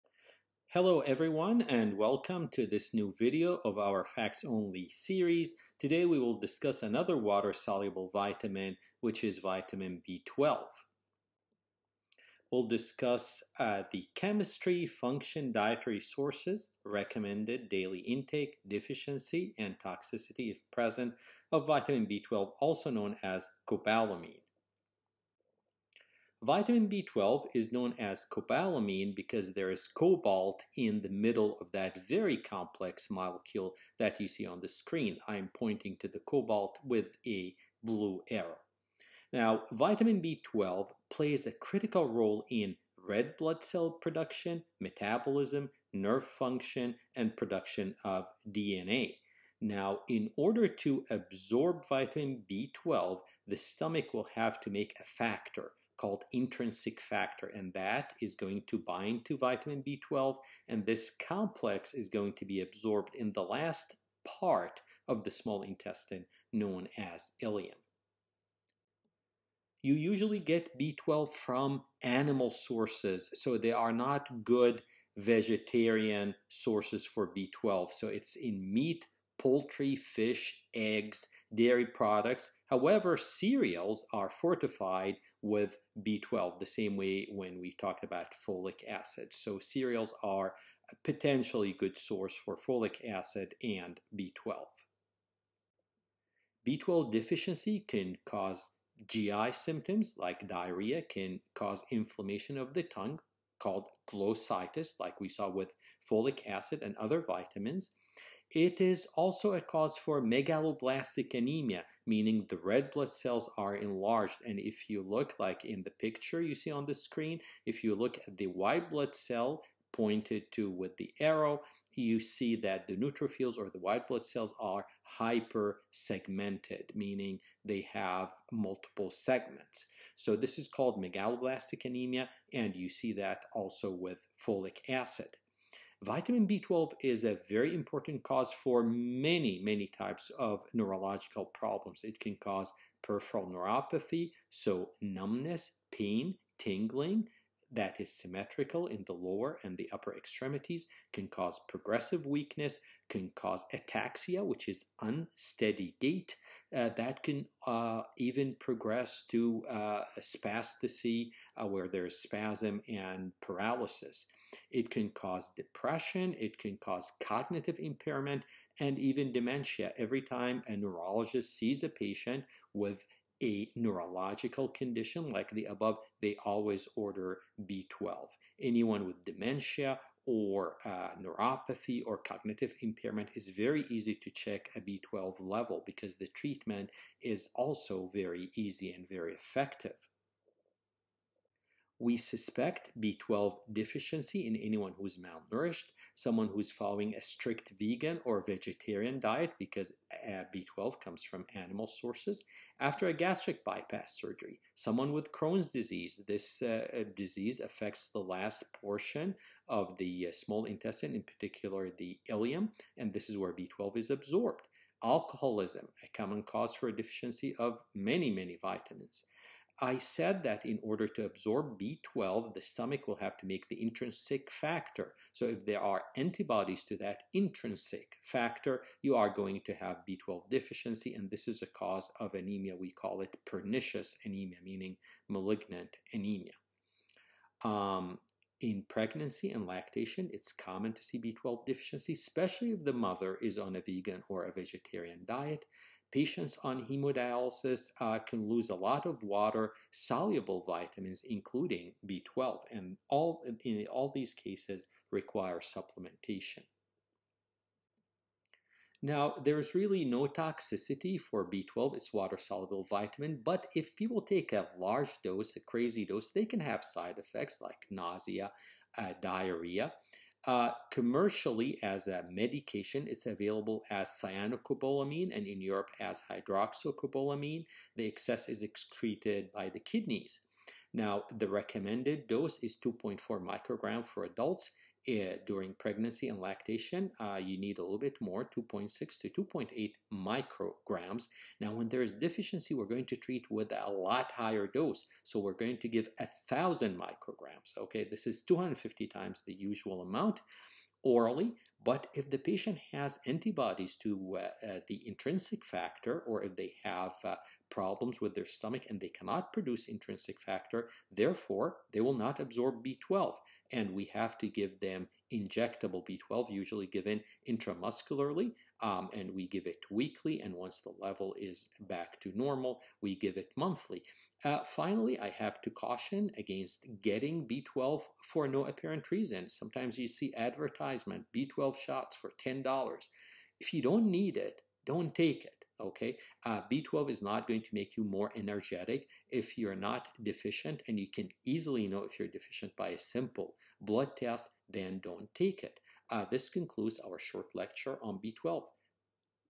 Vitamin B12 (Cobalamin) Facts Only
All you need to know about Vitamin B12 (Cobalamin) in a few minutes. Medical background is not necessary.